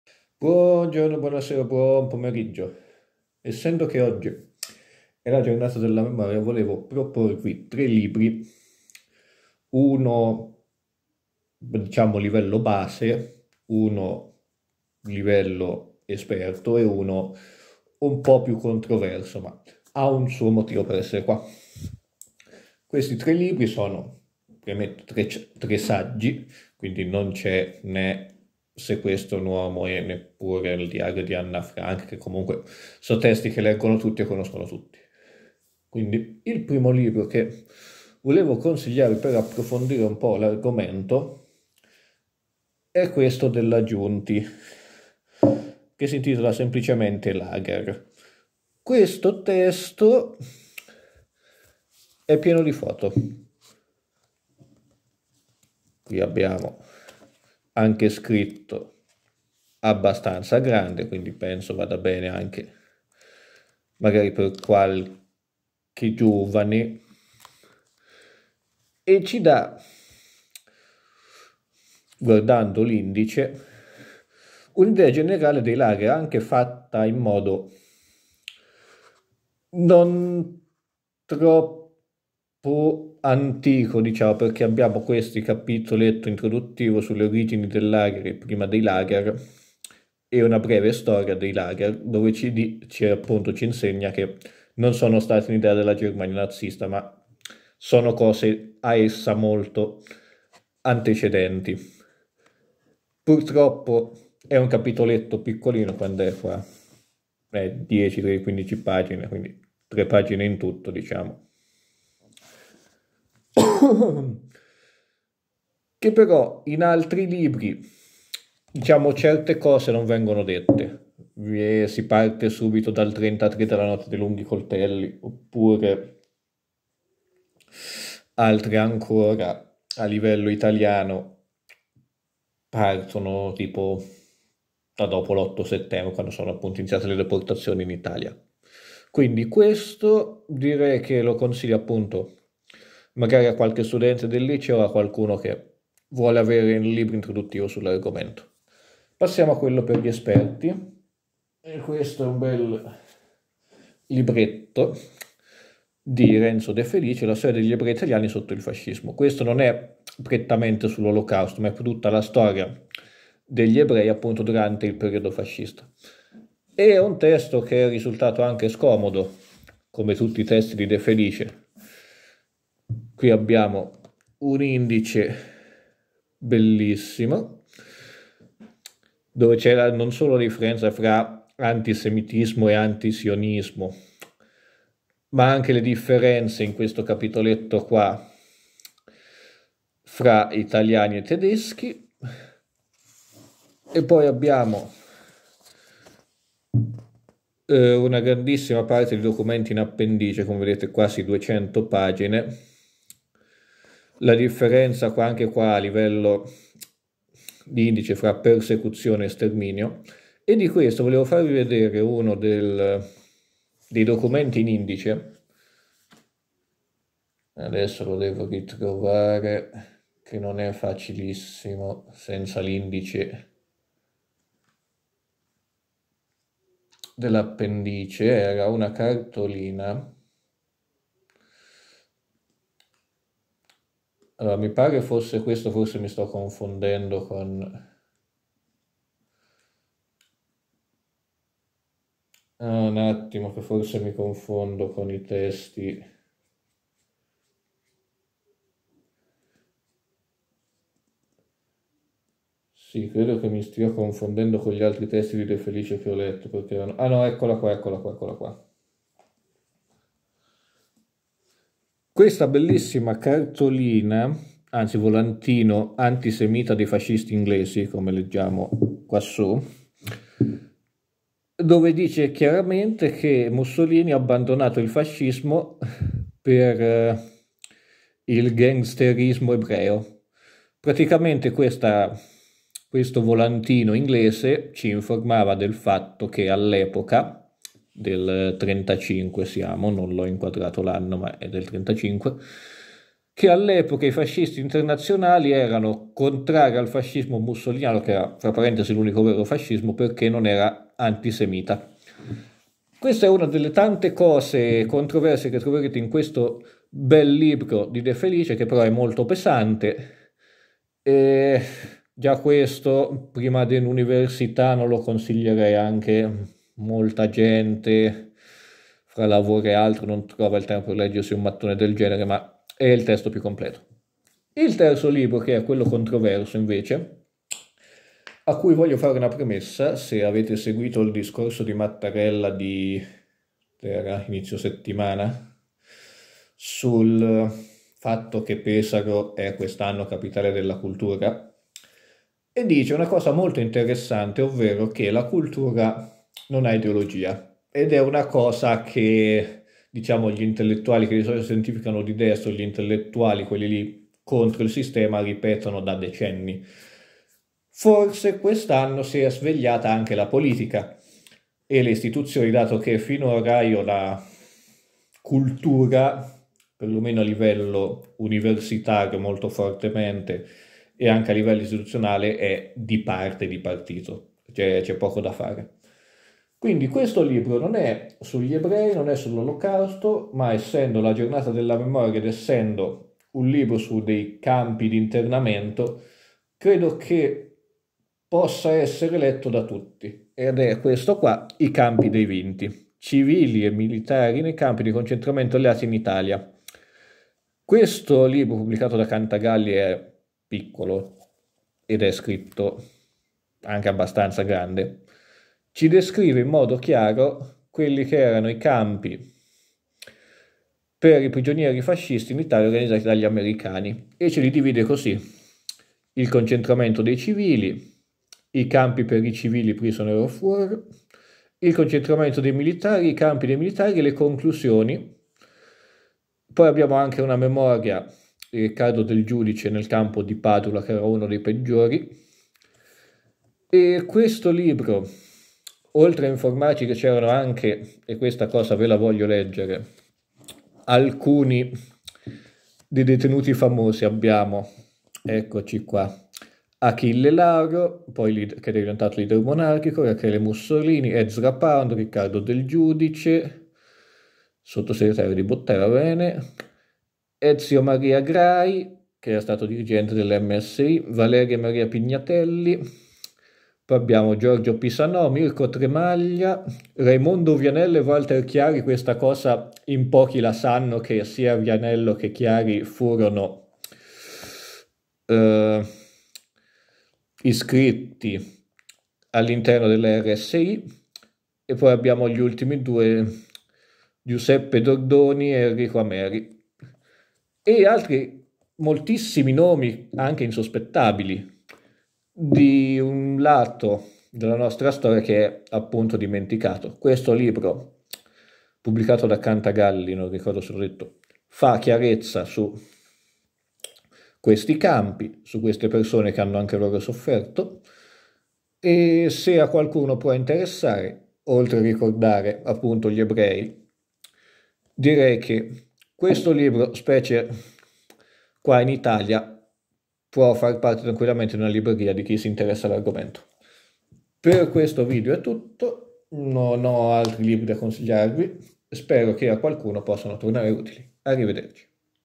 Tre libri per la giornata della memoria
Consiglio questi libri per chiunque voglia approfondire quel periodo storico.